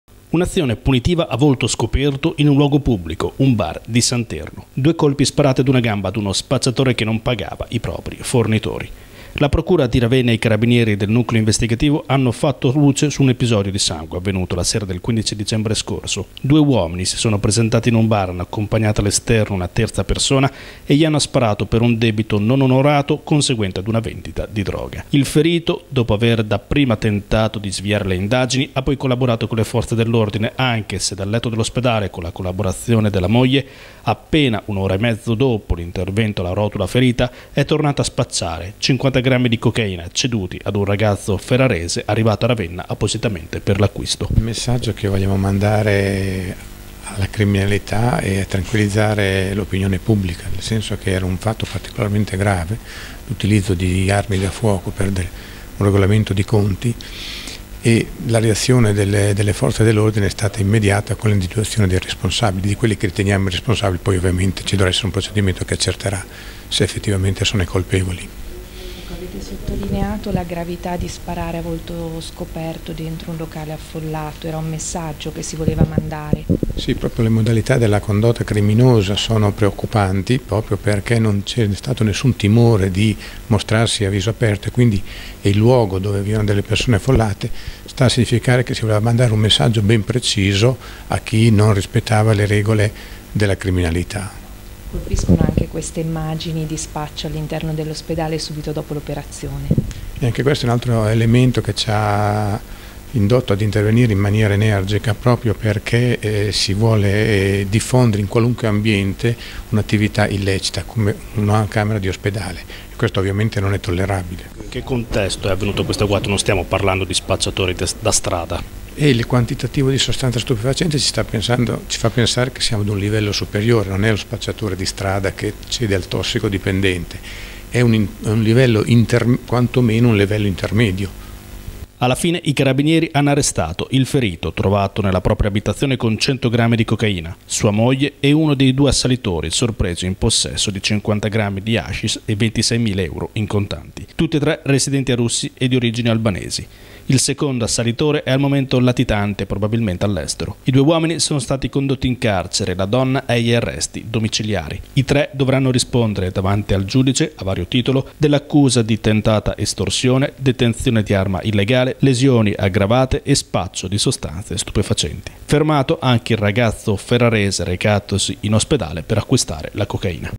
Gambizzato fuori da un bar per un debito di droga. Poi sorpreso a spacciare anche in ospedale
Un’azione punitiva a volto scoperto, in un luogo pubblico, un bar di Santerno. Due colpi sparati ad una gamba ad uno spacciatore che non pagava i propri fornitori. La Procura di Ravenna e i Carabinieri del Nucleo Investigativo hanno fatto luce su un episodio di sangue avvenuto la sera del 15 dicembre scorso. Due uomini si sono presentati in un bar, hanno accompagnato all’esterno una terza persona e gli hanno sparato per un debito non onorato conseguente ad una vendita di droga. Il ferito, dopo aver dapprima tentato di sviare le indagini, ha poi collaborato con le forze dell’ordine anche se dal letto dell’ospedale, con la collaborazione della moglie, appena un’ora e mezzo dopo l’intervento alla rotula ferita, è tornato a spacciare, 50 grammi di cocaina, ceduti ad un ragazzo ferrarese arrivato a Ravenna appositamente per l’acquisto.

Alla fine i Carabinieri hanno arrestato il ferito, trovato, nella propria abitazione, con 100 grammi di cocaina, sua moglie e uno dei due assalitori, sorpreso in possesso di 50 grammi di hashish e 26 mila euro in contanti. Tutti e tre residenti a Russi e di origini albanesi. Il secondo assalitore è al momento latitante, probabilmente all’estero. I due uomini sono stati condotti in carcere, la donna è agli arresti domiciliari. I tre dovranno rispondere, davanti al giudice, a vario titolo, dell’accusa di tentata estorsione, detenzione di arma illegale, lesioni aggravate e spaccio di sostanze stupefacenti. Fermato anche il ragazzo ferrarese recatosi in ospedale per acquistare la cocaina.